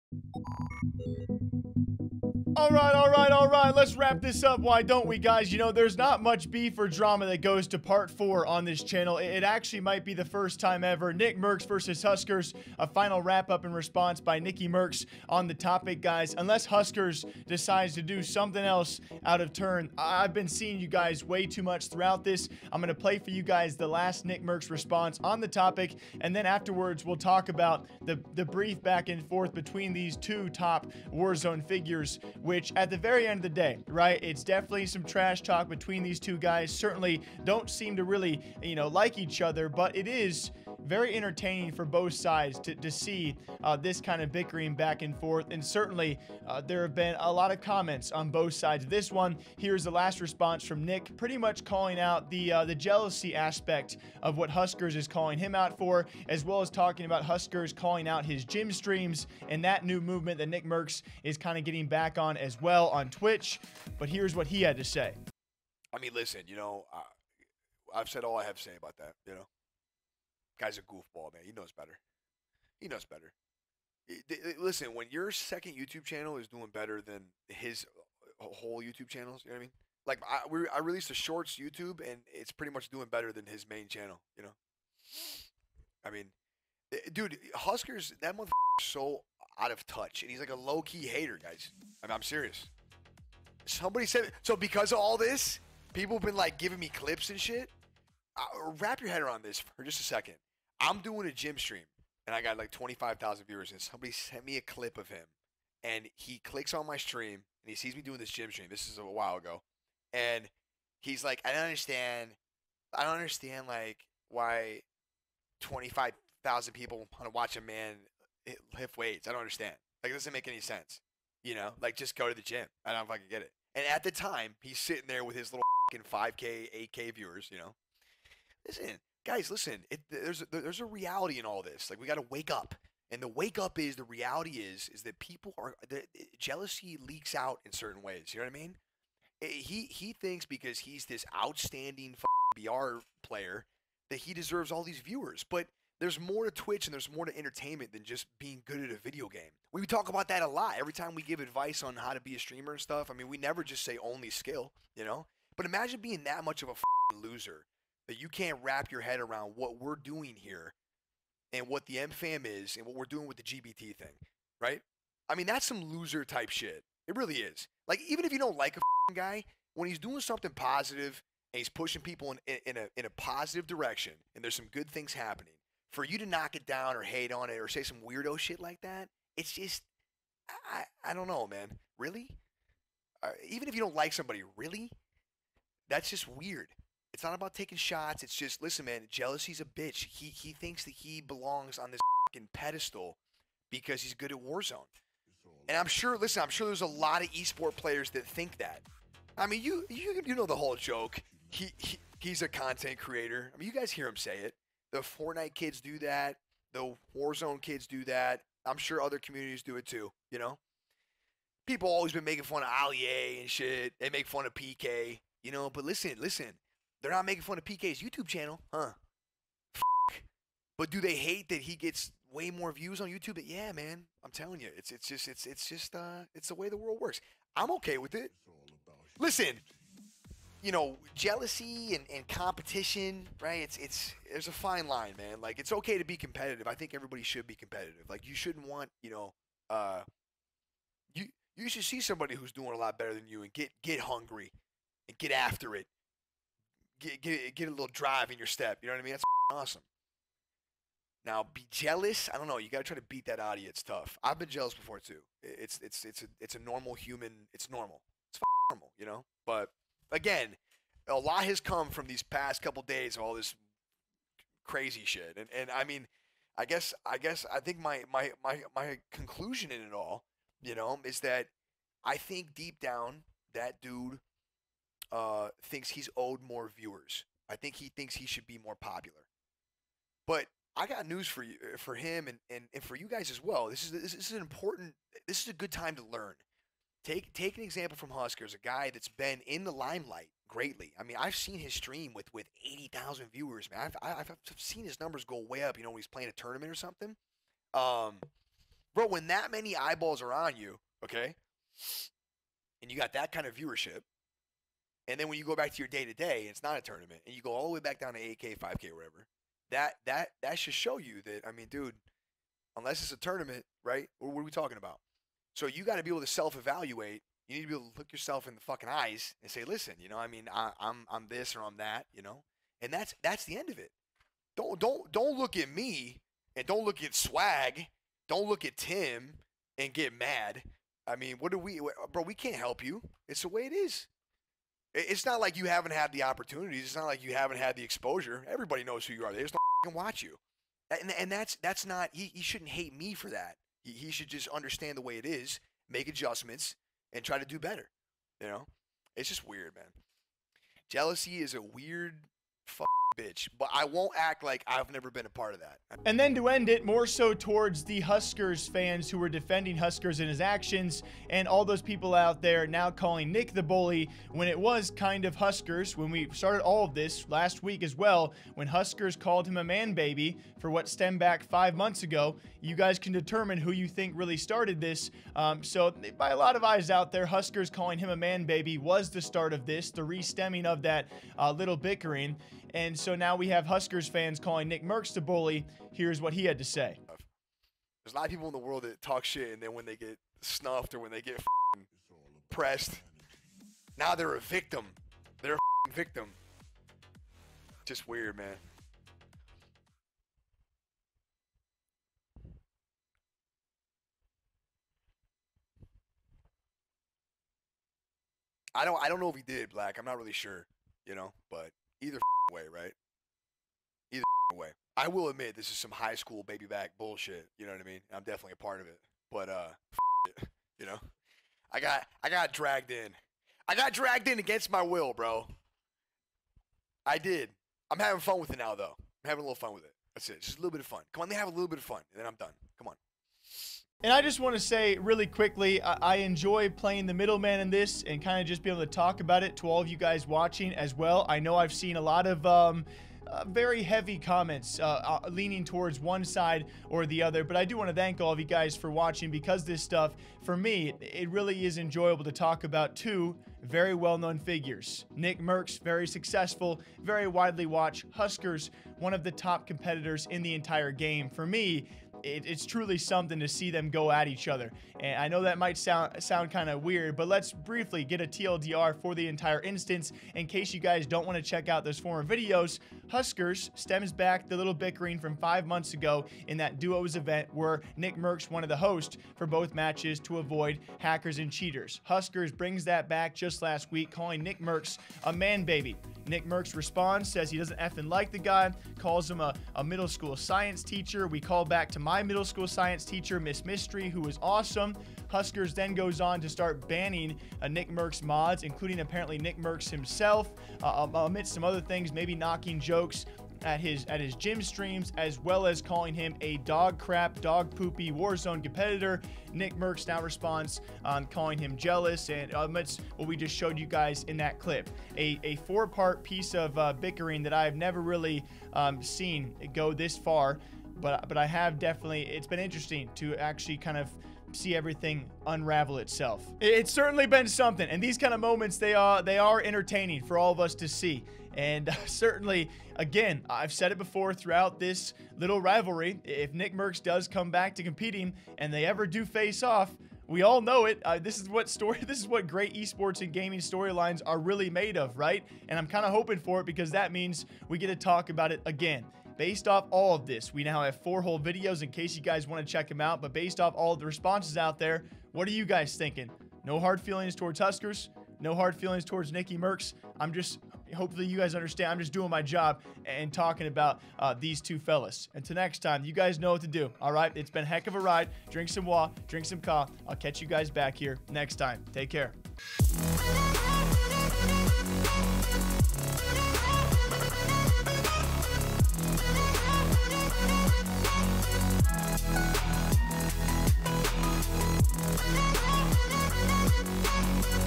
Nickmercs FINAL Response to Huskerrs "Fkin Loser"
After a week of back and forths it's @NICKMERCS giving his final response to @HusKerrs on their Warzone drama.